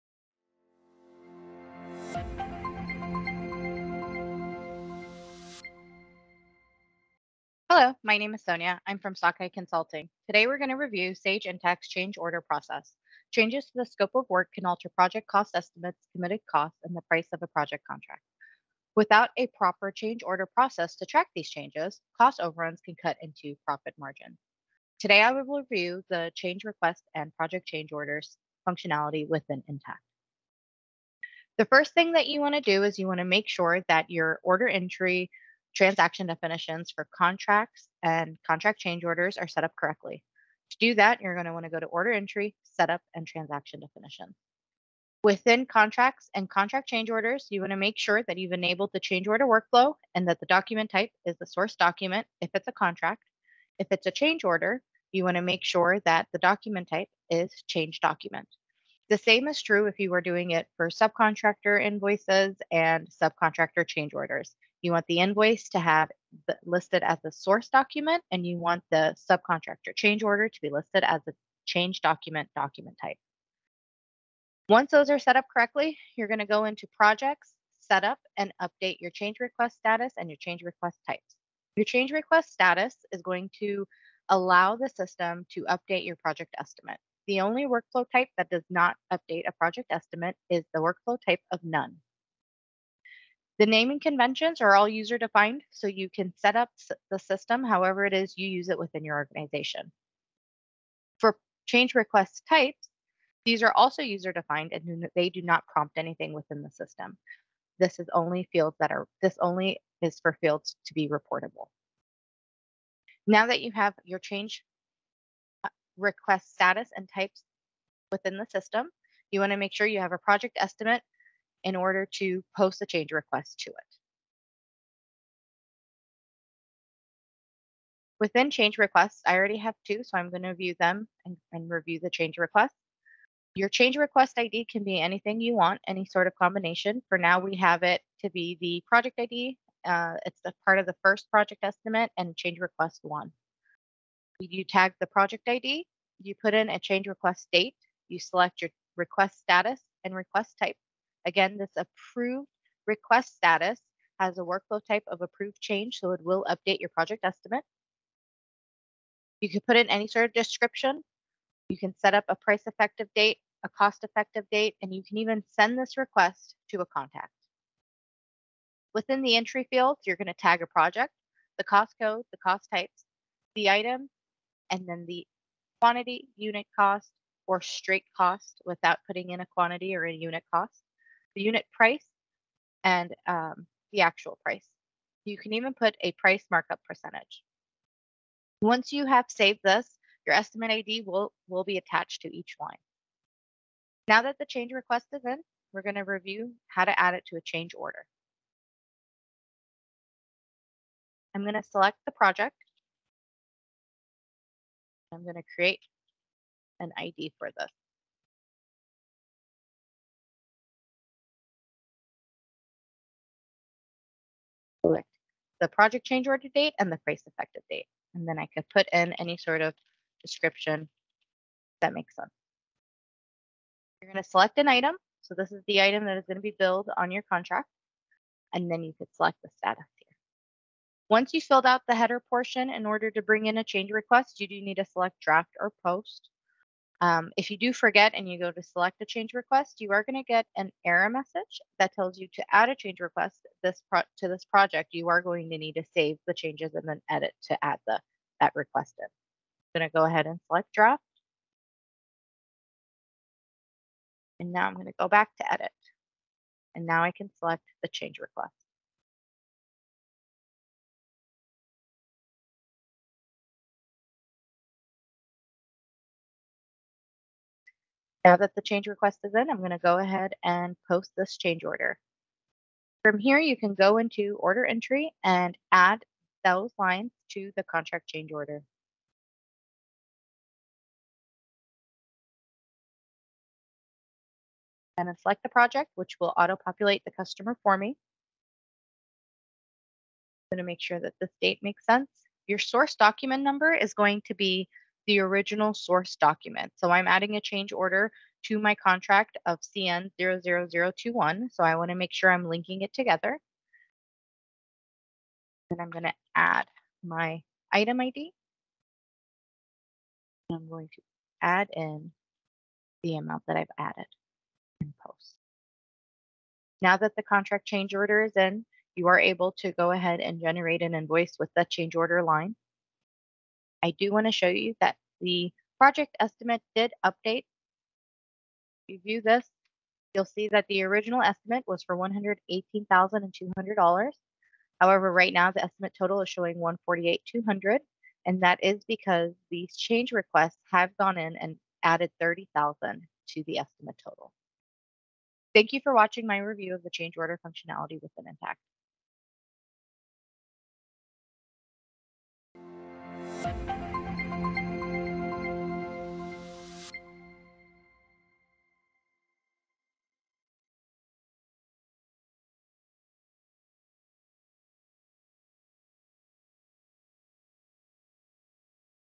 Change Order Process in Sage Intacct
In this video, our Senior ERP Consultant, Sonia Parsa, reviews the Sage Intacct order process and the importance of making proper changes in order to avoid cutting into the profit margin.

Sockeye is a relationship-forward firm that leverages Sage Intacct to effectively manage the complex demands for your company.